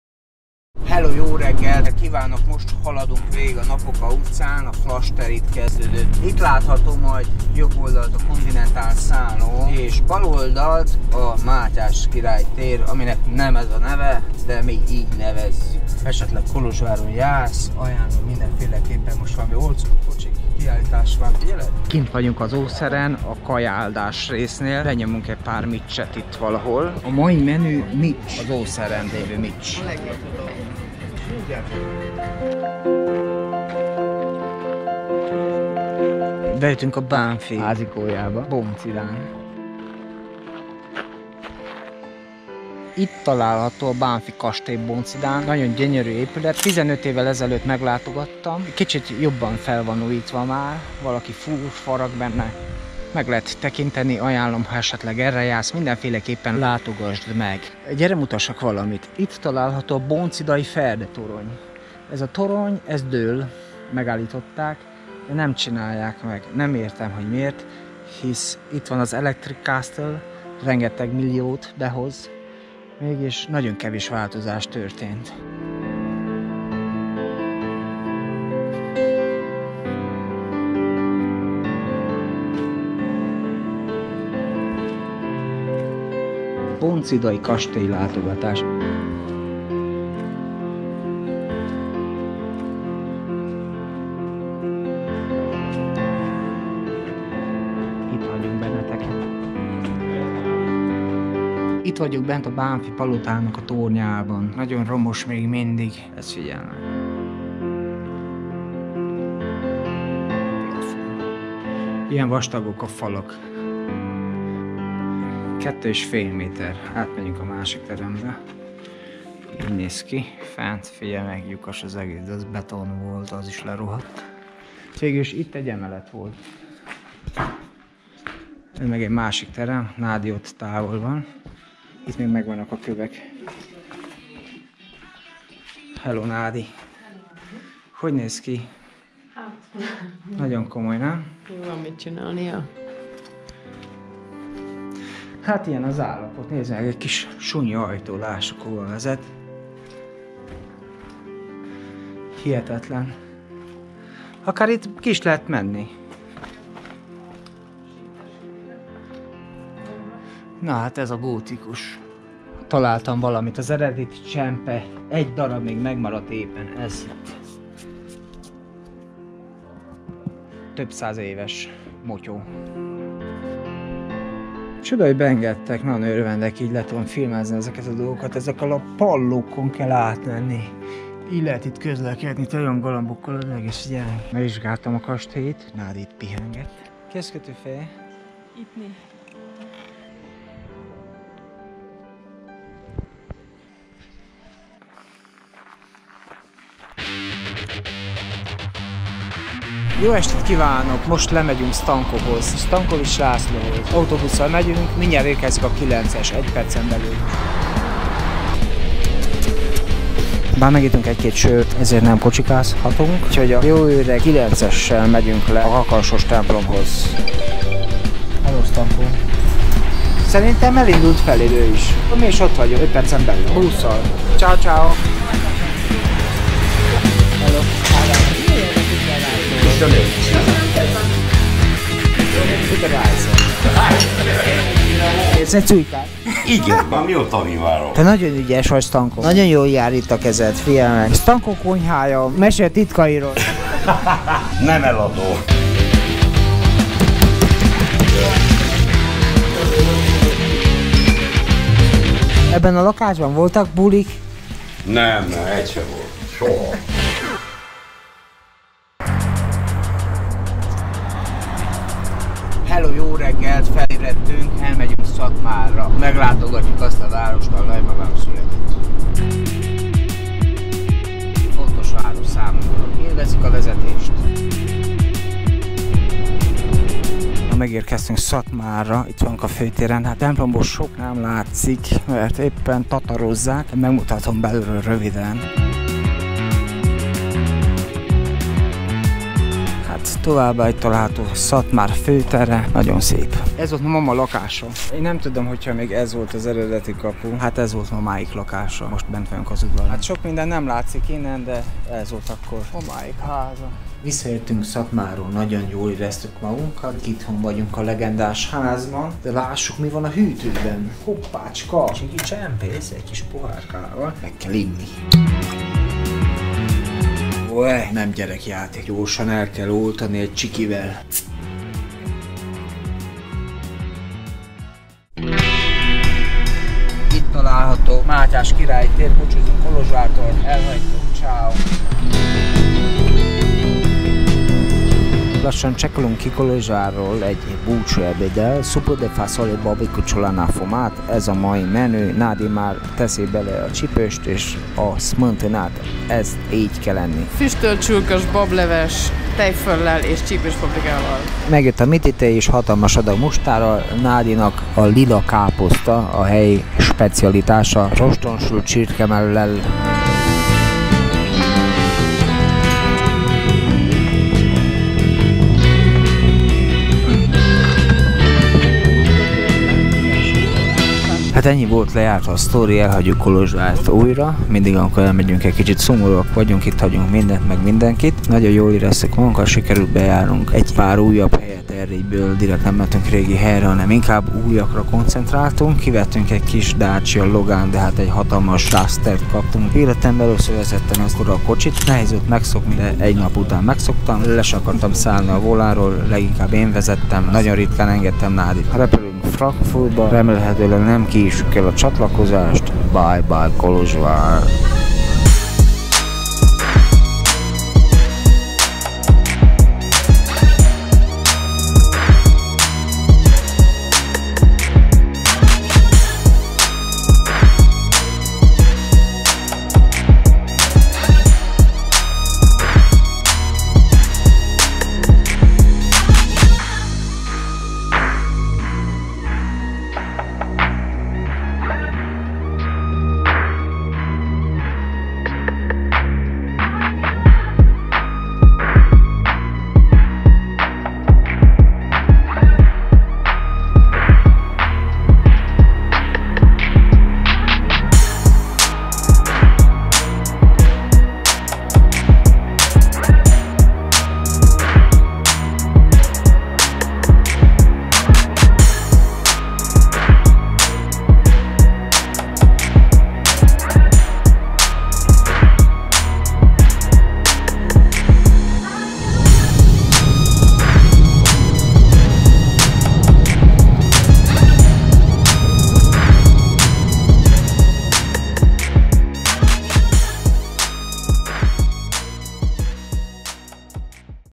Rövid erdélyi túra: kicsekkolás (Ádám Vlog)
Ez egy vlog. 2019 októberében hazamentünk pár napra Kolozsvárra Nádi születésnapját megünnepelni. Ezt az utat megörökítettem egy vlog formájában. Fogadjátok szeretettel.

In October 2019, we went home to Kolozsvár for a few days to celebrate Nádi's birthday. I recorded this trip in a vlog. Enjoy!